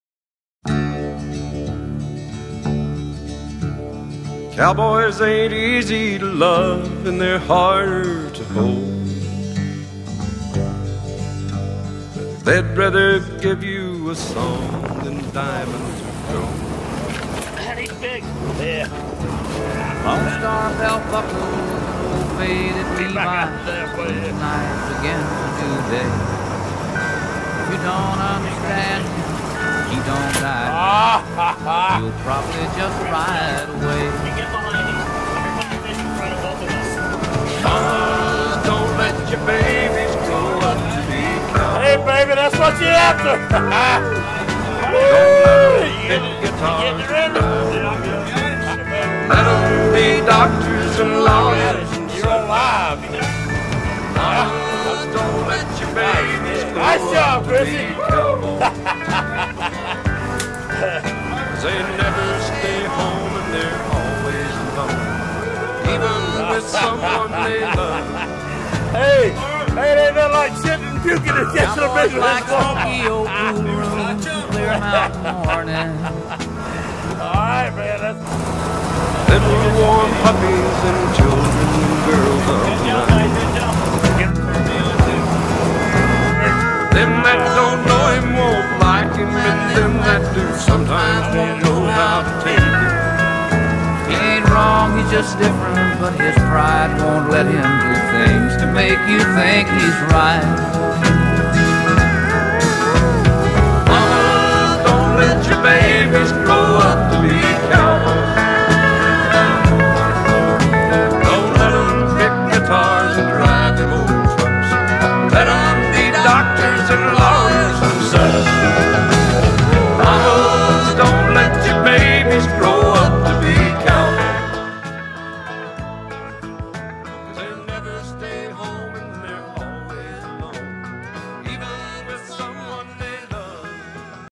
Fishing Key West with Cowboy Charters-Key West-Sailfish with Amos and crew
Off Shore fishing in Key West with Sharky. Amazing weather and lots of fun catching sailfish kingfish and bonita.